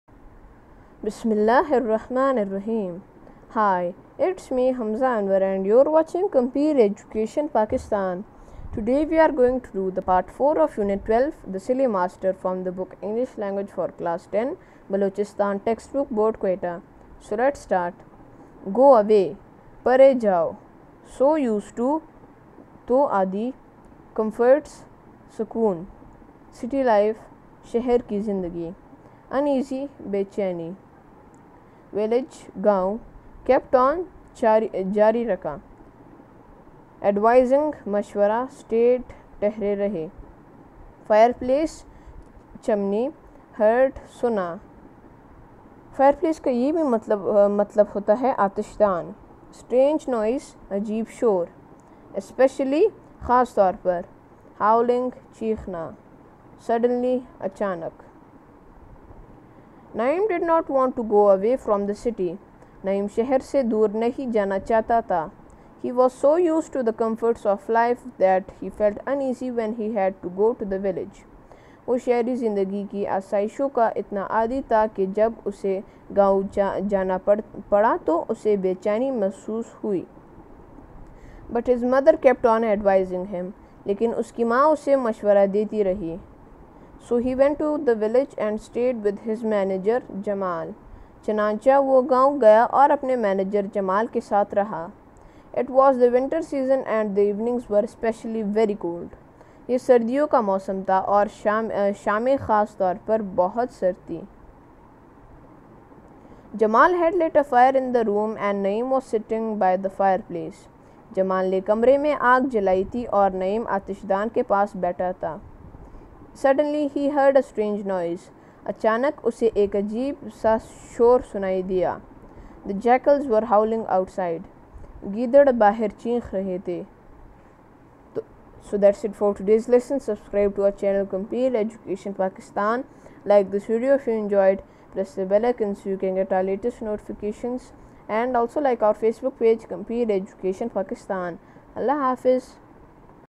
Unit 12, A Silly Master, Translation and Word Meanings, Part 4, Grade 10, BTBQ
Unit 12
The Silly Master
Grade 10

go away پرے جاؤ
so used to تو عادی
comforts  سکون
city life شہر کی زندگی
uneasy  بے چینی
village گاؤں
kept on جاری رکھا
advising مشورہ
stayed  ٹھہرے رہے
fireplace.  چمنی.
heard سنا
strange noise عجیب شور
especially  خاص طور پر
howling چیخنا
Suddenly اچانک۔
Naeem did not want to go away from the city. نعیم شہر سے دور نہیں جانا چاہتا تھا۔
 He was so used to the comforts of city life that he felt uneasy when he had to go to the village.   وہ شہری زندگی کی آسائشوں کا اتنا عادی تھا کہ جب اسے گاؤں جانا پڑا تو اسے بے چینی محسوس ہوئی۔
 But his mother kept on advising him.   لیکن اس کی ماں اسے مشورہ دیتی رہی۔
 So he went to the village and stayed with his manager, Jamal.    چنانچہ وہ گاؤں گیا اور اپنے منیجر جمال کے ساتھ رہا۔
It was the winter season and the evenings were especially very cold.  یہ سردیوں کا موسم تھا اور شامیں خاص طور پر بہت سرد تھیں۔
Jamal had lit a fire in the room and Naeem was sitting by the fireplace.  جمال نے کمرے میں آگ جلائی تھی اور نعیم آتشدان کے پاس بیٹھا تھا۔
Suddenly he heard a strange noise. اچانک اسے ایک عجیب سا شور سنائی دیا۔
 The jackals were howling outside   گیدڑ باہر چیخ رہے تھے۔